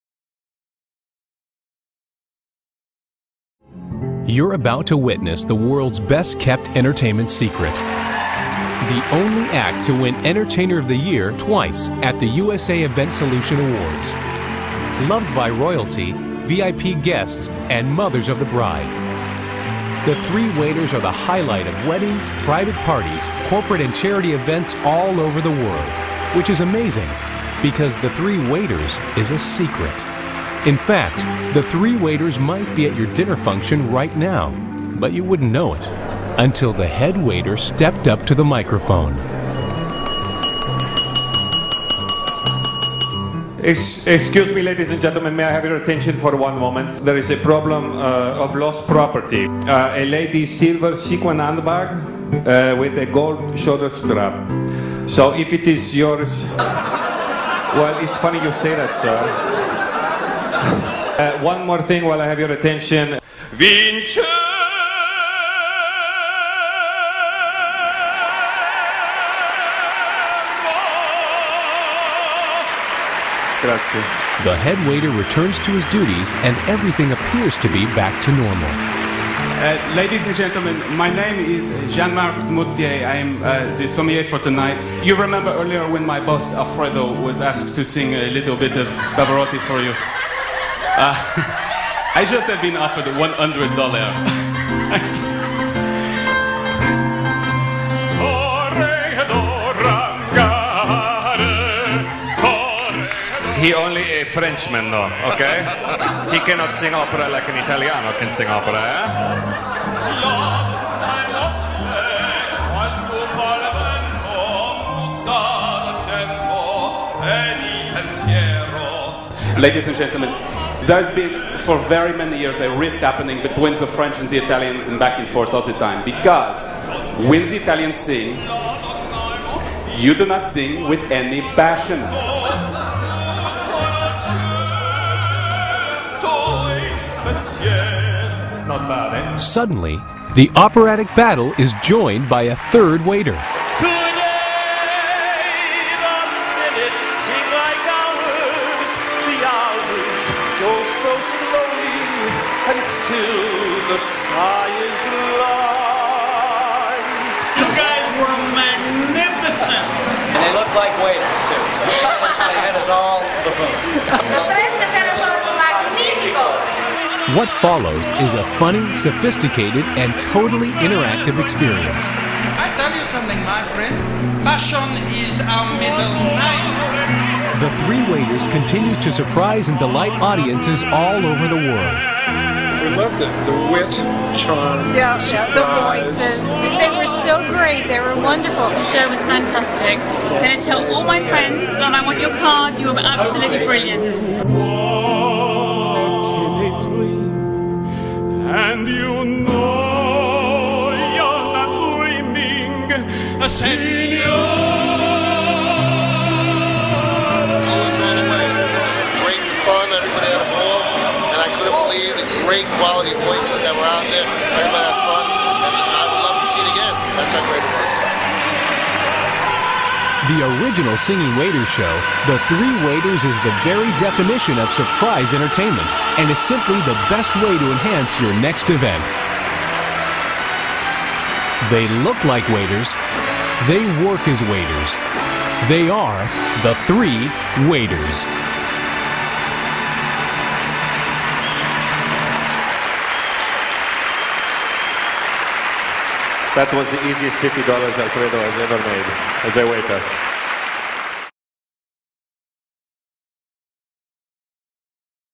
The Three Waiters
Singing waiters in the guise of ordinary waiters, deliver a show full of humor, sophistication and great surprises. More than just a singing waiters show.